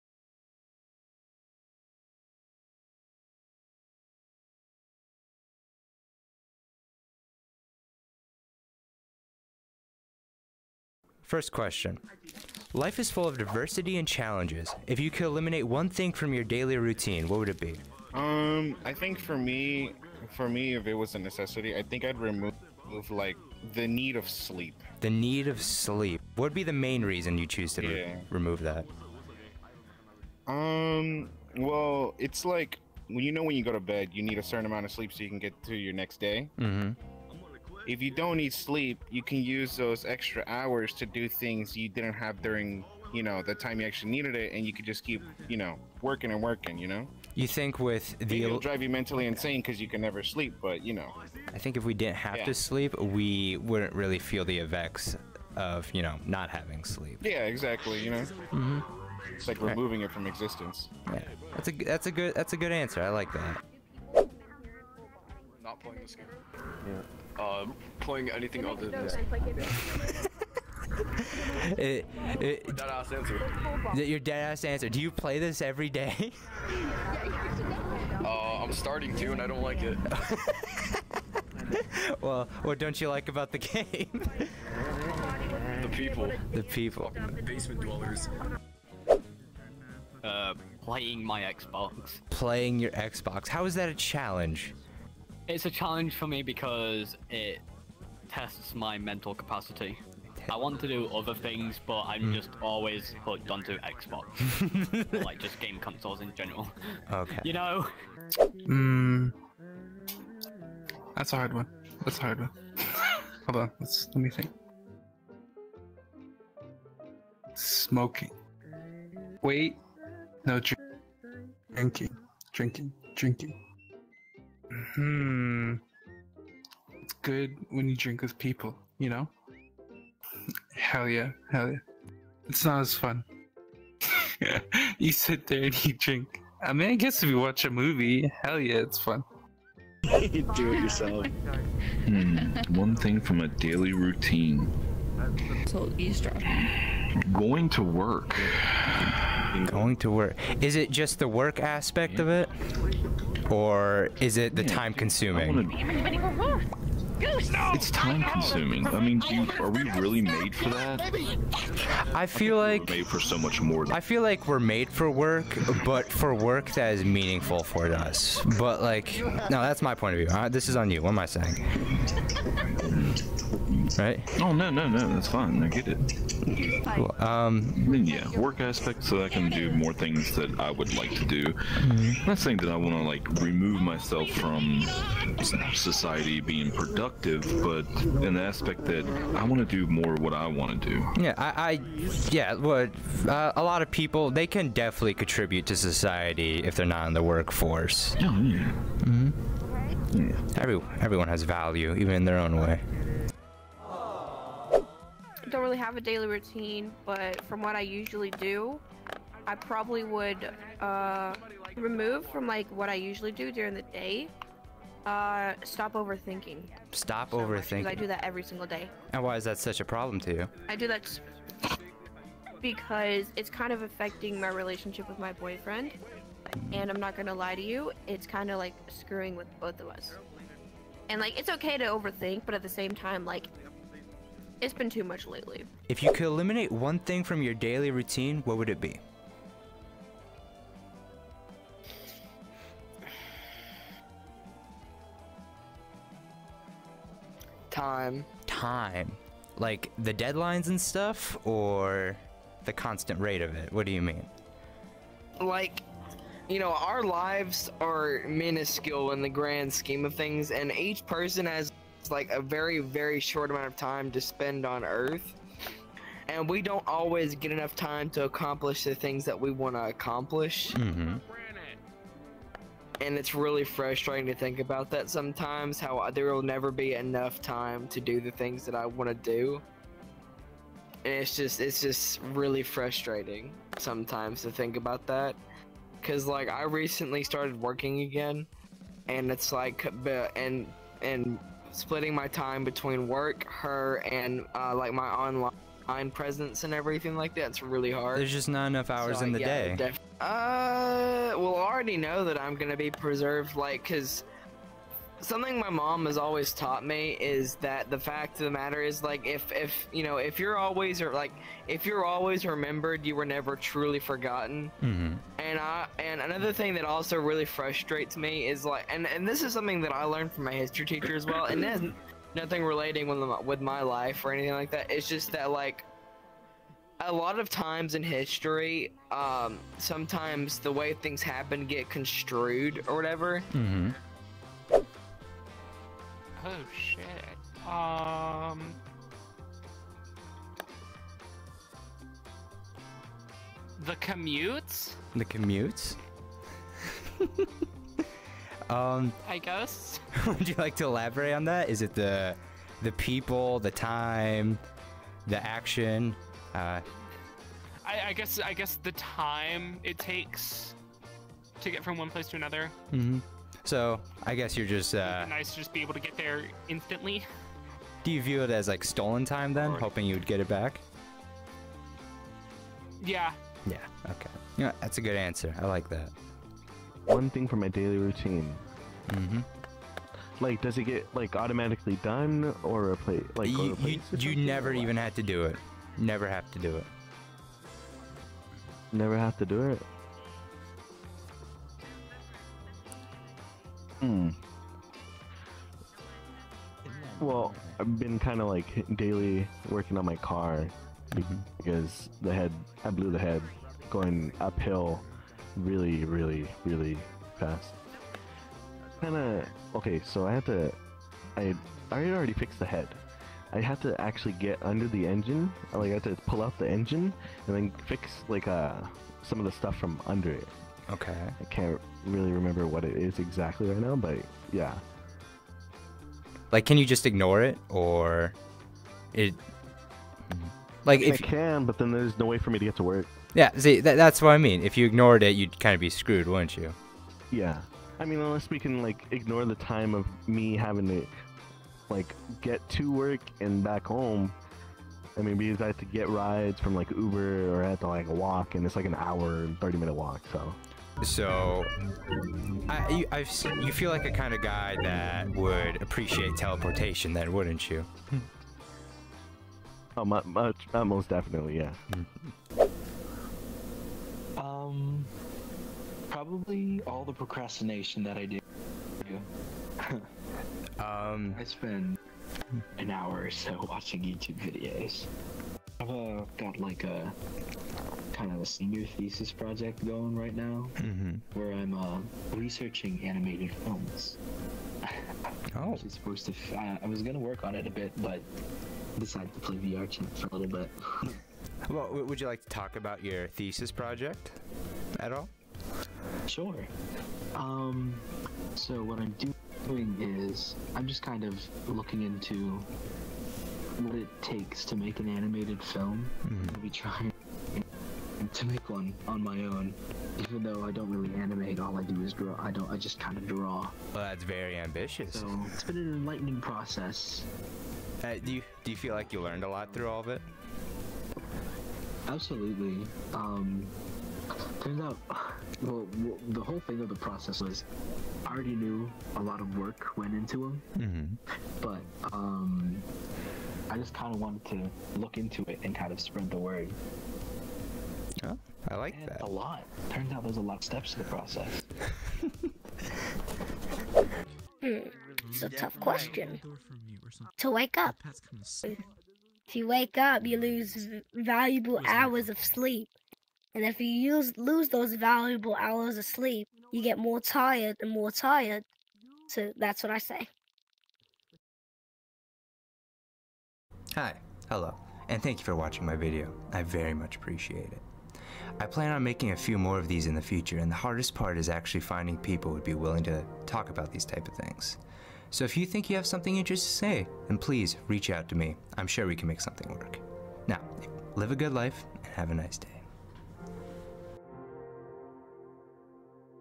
Small Talks - What would you remove from your daily routine?
thank you to all these wonderful people who were willing to have a chat with me.
Sorry to whopper guy, i forgot to hit record when we're talking ;_;